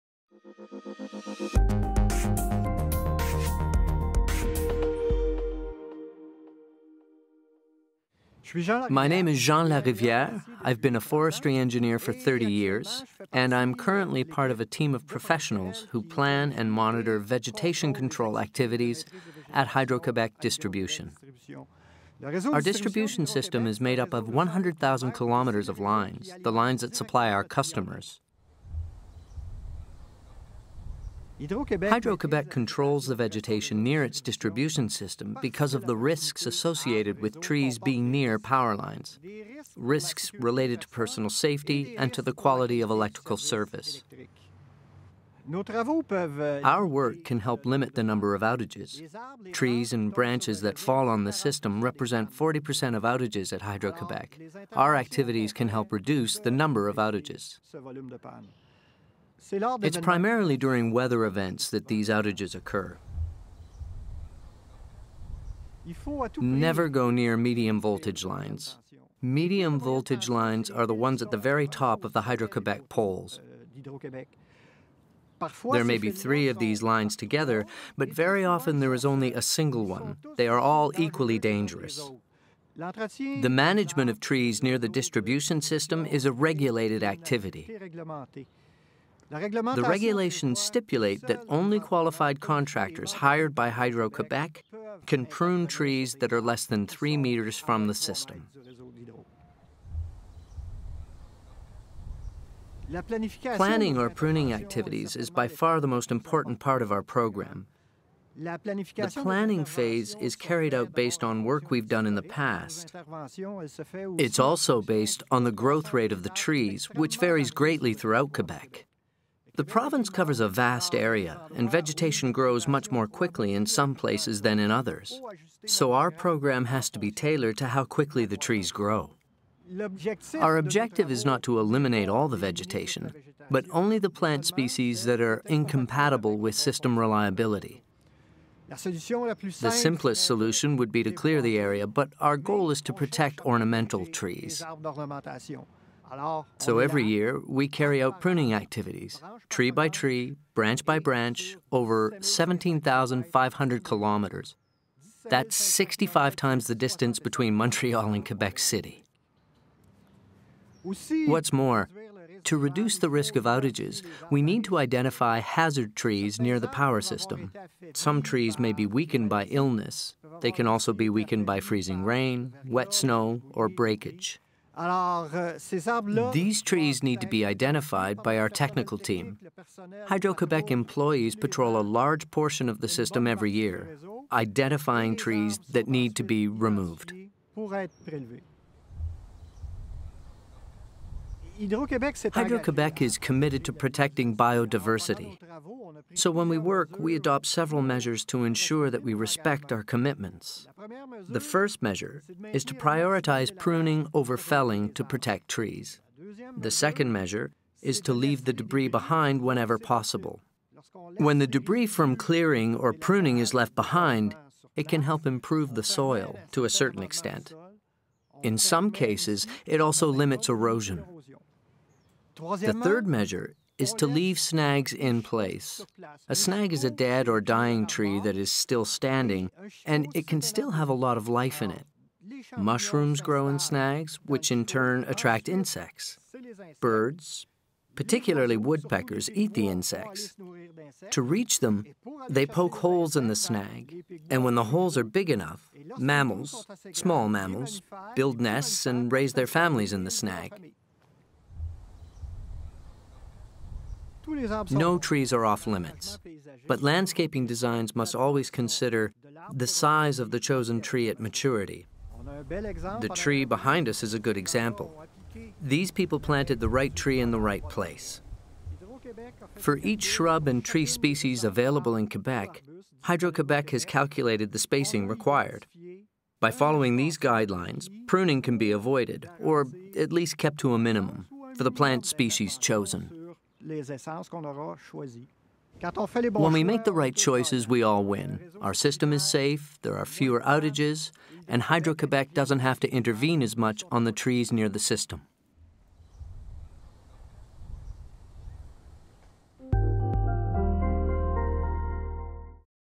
Controlling vegetation near the power system: Expert views
To ensure public and worker safety, prevent outages and optimize power system operation, Hydro-Québec maintains a clearance distance between its power lines and vegetation located near the distribution network, which supplies electricity to homes.

The company favors vegetation management methods that respect the natural environment and promote biodiversity close to its lines. The objective of Hydro-Québec's work is not to eliminate all the vegetation at a given site but only the plant species that are incompatible with system or equipment reliability.

Vegetation-control work is planned by Hydro-Québec's forestry engineers and carried out by qualified contractors hired by Hydro-Québec.